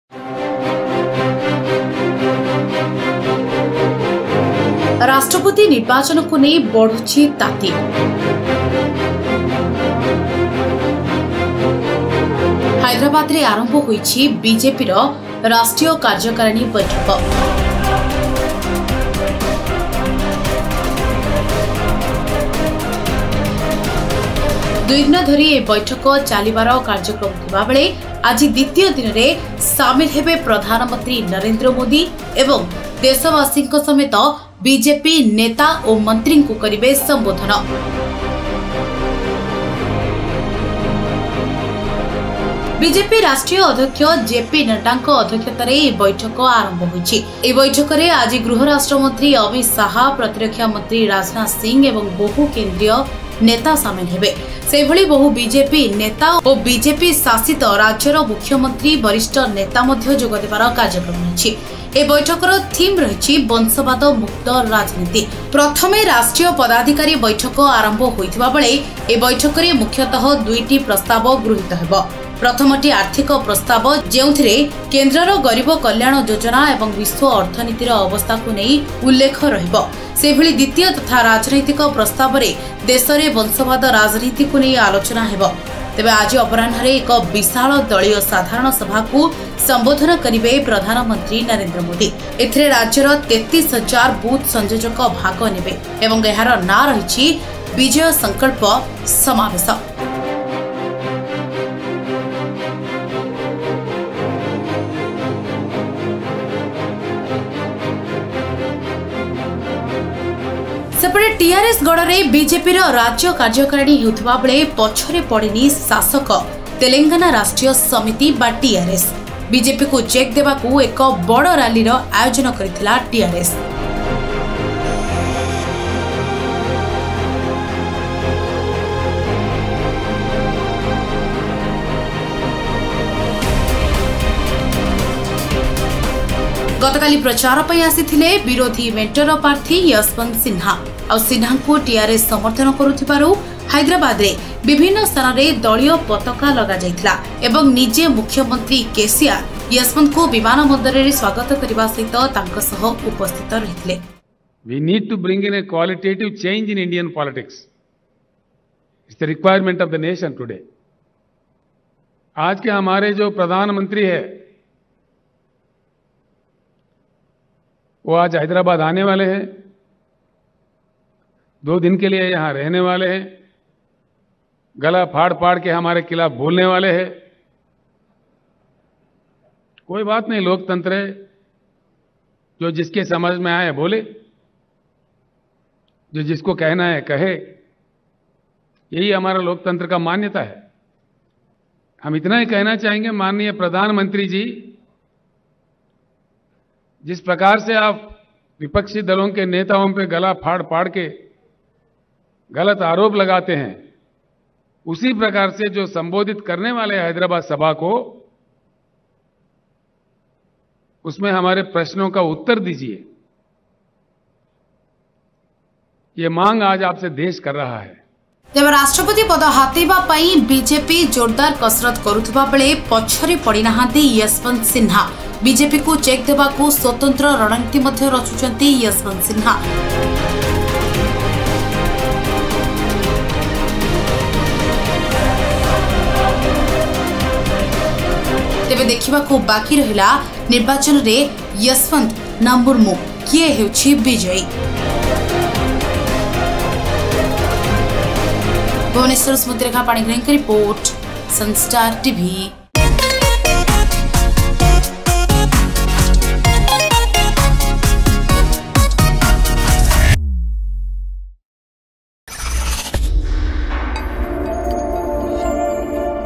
TRS ଗଡ଼ ରେ ବିଜେପିର କାର୍ଯ୍ୟକାରିଣୀ ବୈଠକ, ଚୁପ ରହିଲାନି TRS ବିଶାଳ ଶୋଭାଯାତ୍ରା କରି ବିଜେପିକୁ ଦେଲା ଚେକ
TRS ଗଡ଼ ରେ ବିଜେପିର କାର୍ଯ୍ୟକାରିଣୀ ବୈଠକ, ଚୁପ ରହିଲାନି TRS ବିଶାଳ ଶୋଭାଯାତ୍ରା କରି ବିଜେପିକୁ ଦେଲା ଚେକ, ପ୍ରଚାର ପାଇଁ ଆସିଲେ ବିରୋଧୀ ମେଣ୍ଟ ପ୍ରାର୍ଥୀ ଯଶବନ୍ତ ସିହ୍ନା